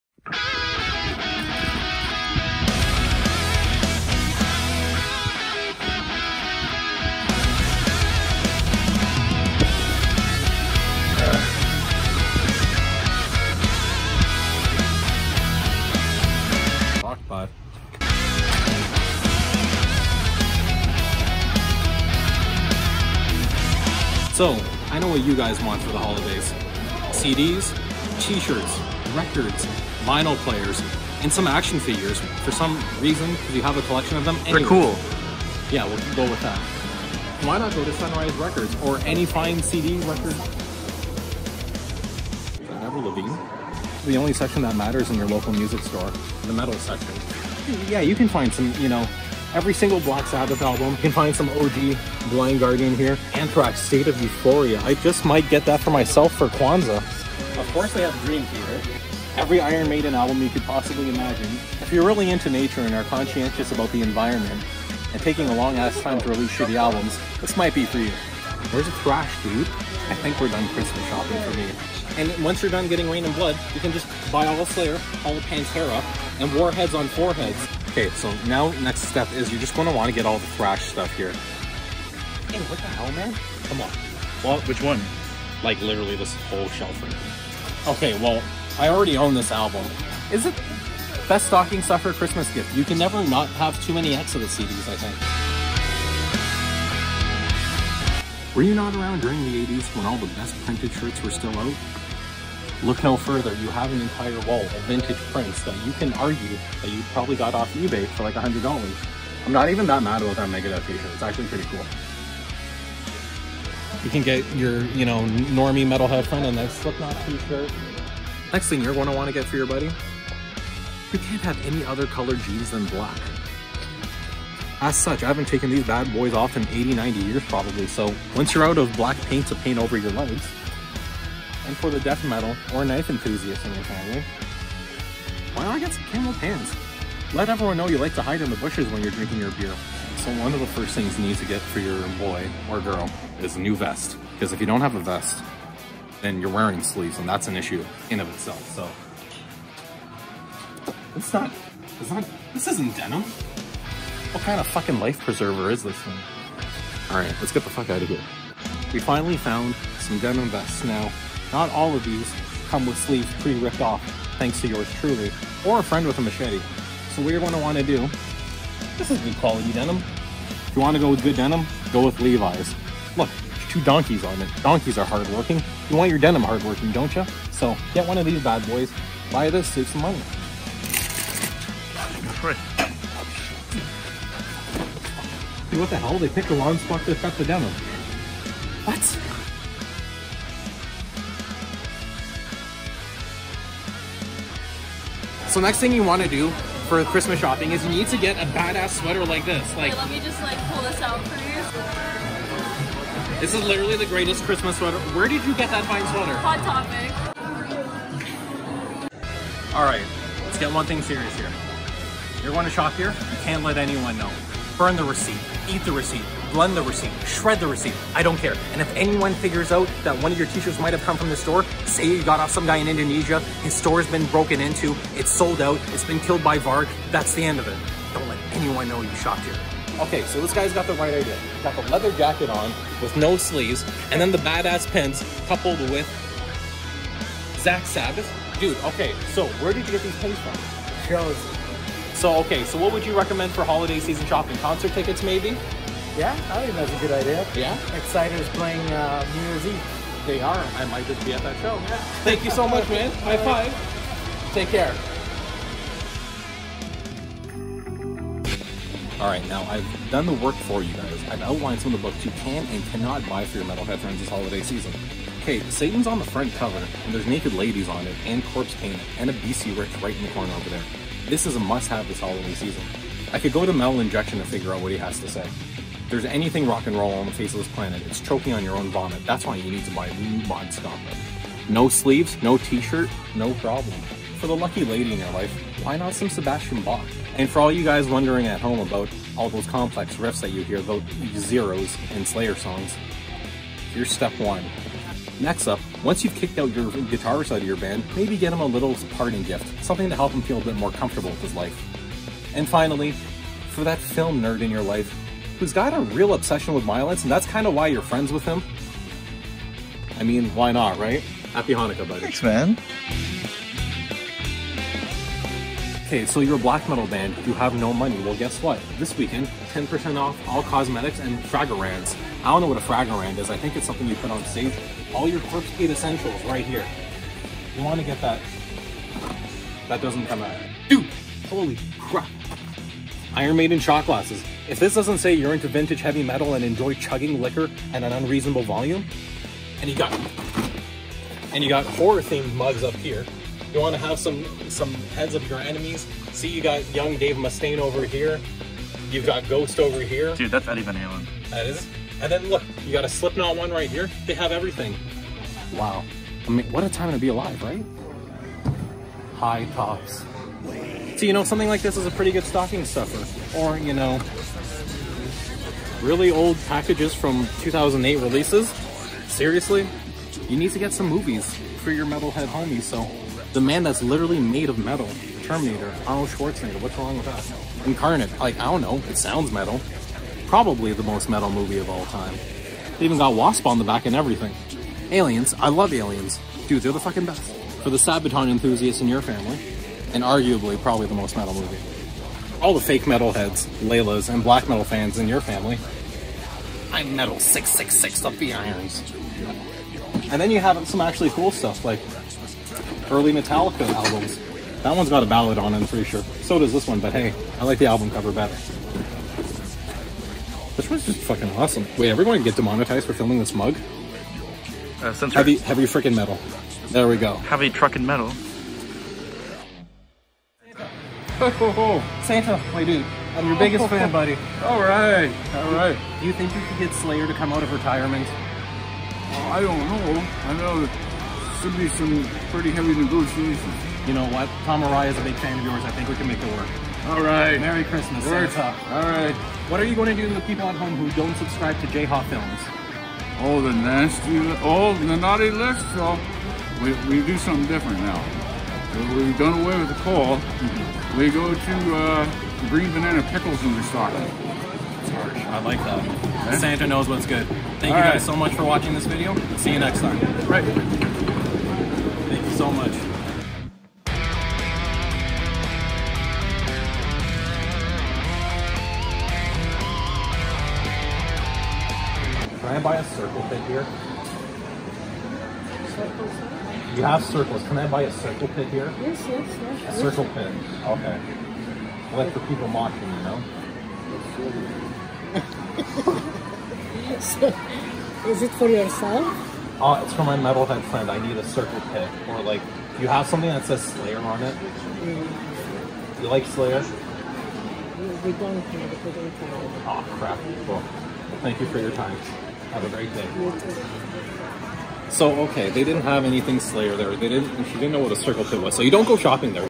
Christmas Shopping For Metalheads
Need some last second holiday gifts for Metalheads? Chris teaches you what to look for when Christmas shopping for your metalhead friends!

Follow us on shit!